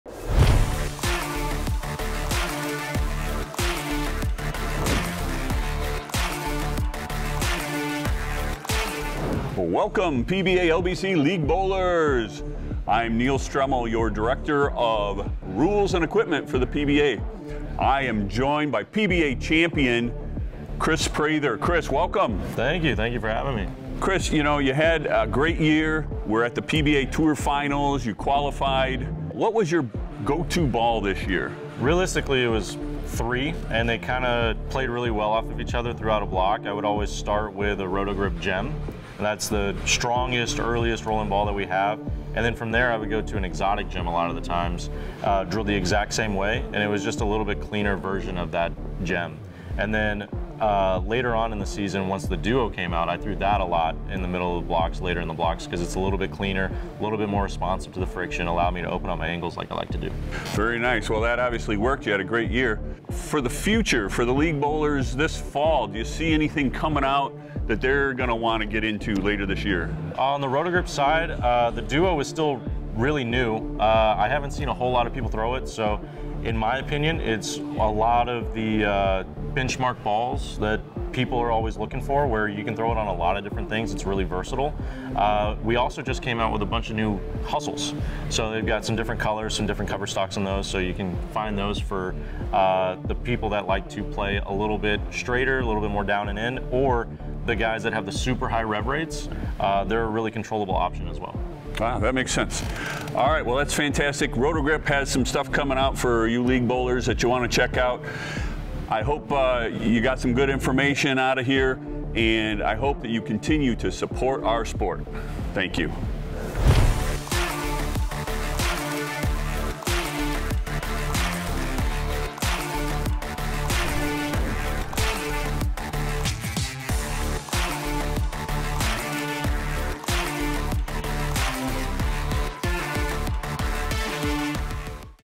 Roto Grip with Kris Prather | Neil Stremmel's Bowling Breakdown
Neil Stremmel and Kris Prather discuss Roto Grip bowling balls and thoughts for ball selections in the future.